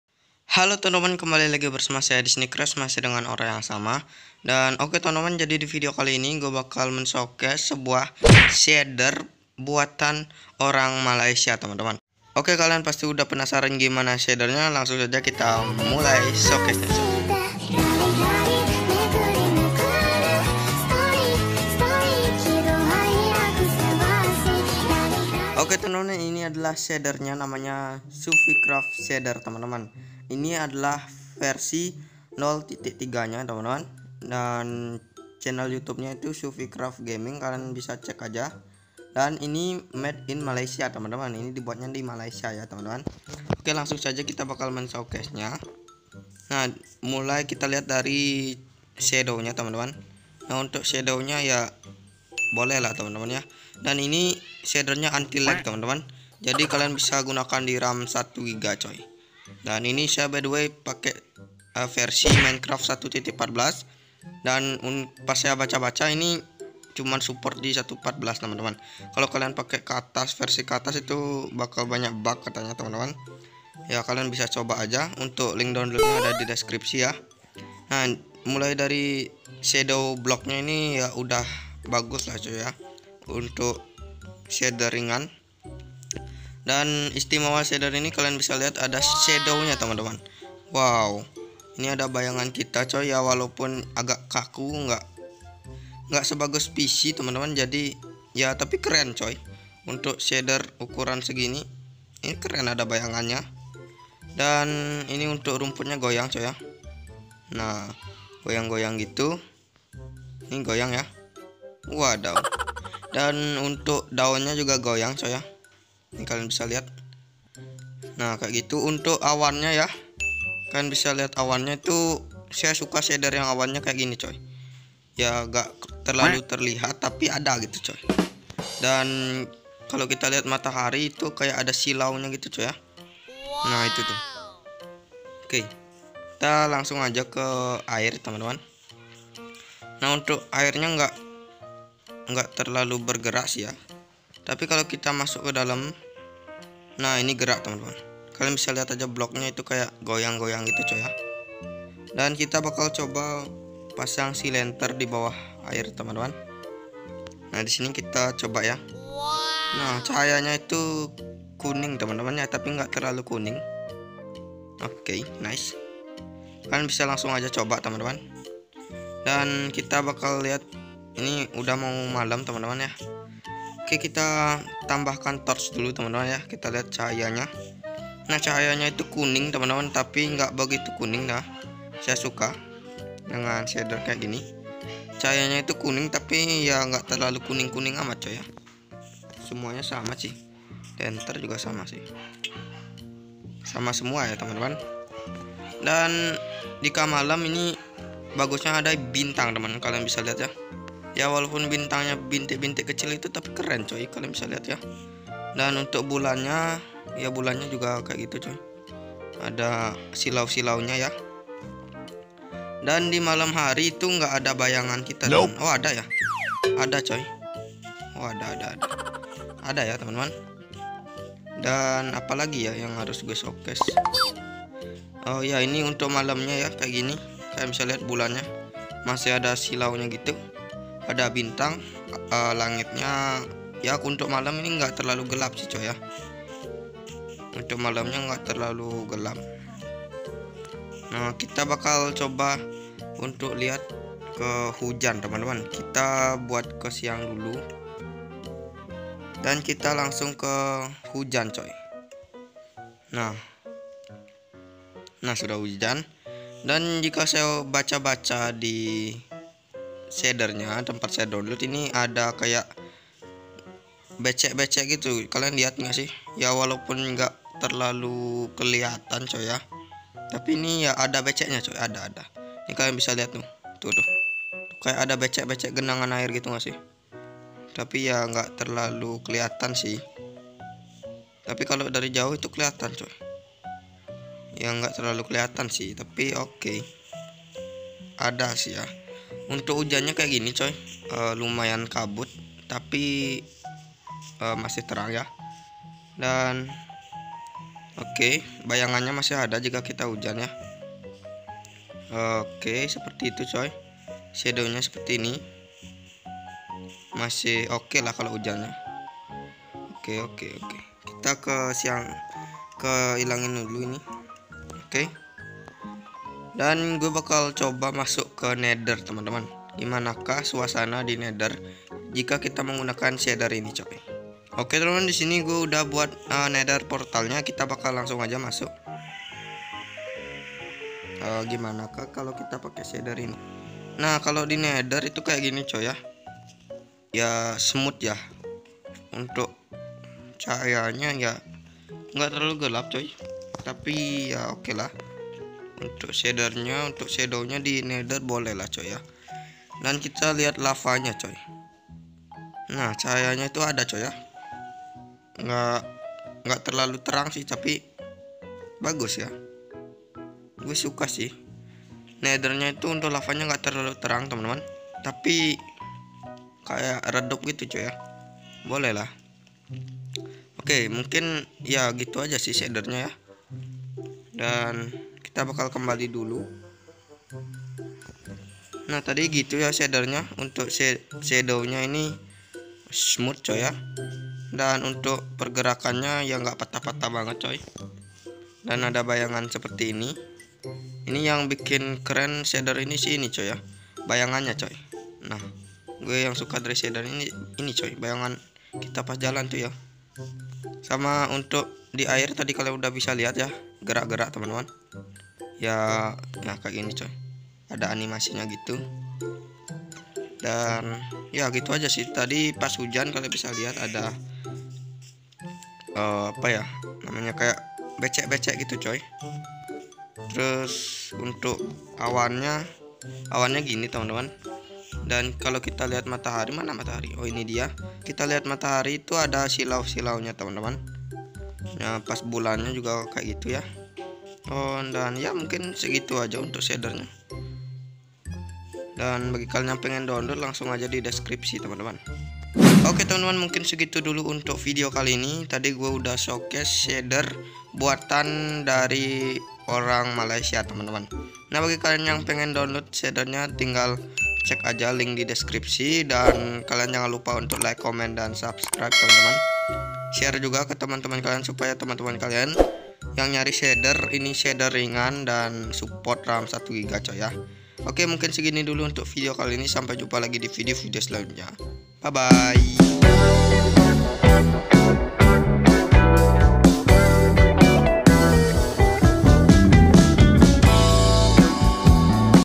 SHADER RINGAN BUATAN MALAYSIA!! SUPORT RAM 1 GB 😱
꧁༃𝒄𝒓𝒆𝒛𝒛༃꧂

«⚠️BUDAYAKAN BACA DESKRIPSI⚠️»
Yoo.. Hallo CrezzTeam wellcome back with me!! Di video kali ini sy akan showcase shader buatan malaysia, suport ram 1 gb!! langsung Download dibawah!

                           °TERIMAKASIH°